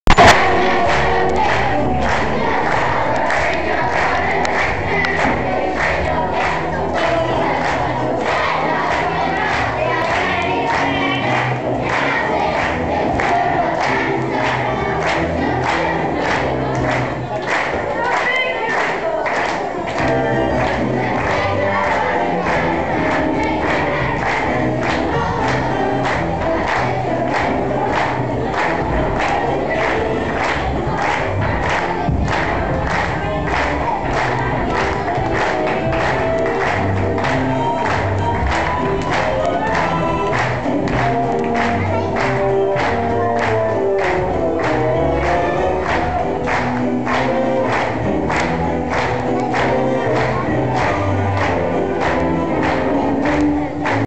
Aladdin :]
When St.Mark's Primary Did Aladdin On The 28/06/06

:D